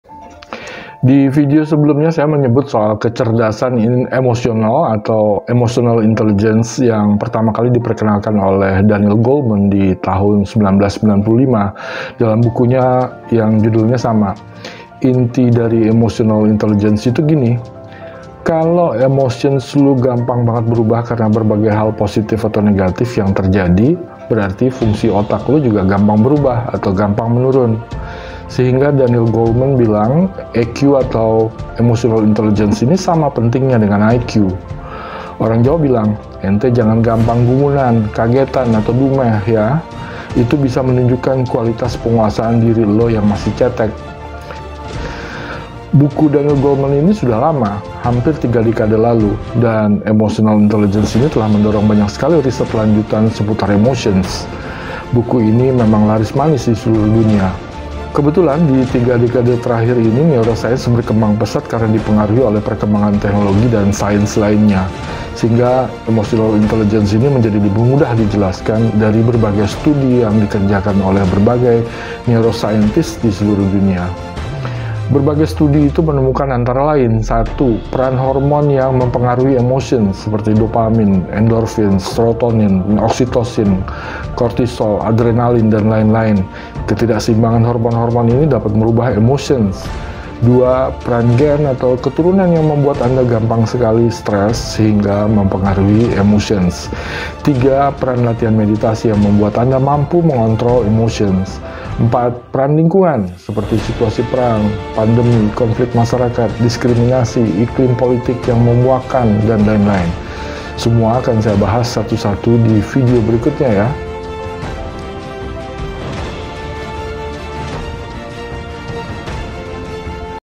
Pentingnya Capres Memiliki Emotional Intelligence
Emotions itu udah lama banget diteliti oleh para saintis. Emotions bukan soal yang ghaib atau sulit dipahami. Memahami bagaikan emotions terbentuk, bakal membantu Anda lebih mudah menjalani hidup, bahkan bisa menjadi pendorong sukses.